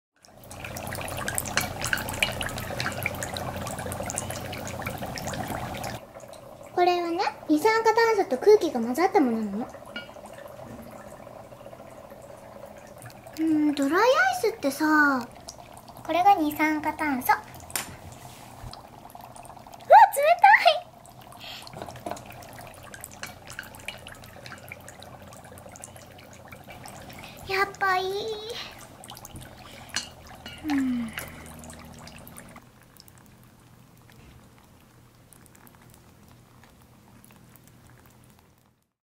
Dry Ice!
Mai's experiment with some dry ice that showed up with a package of frozen goods.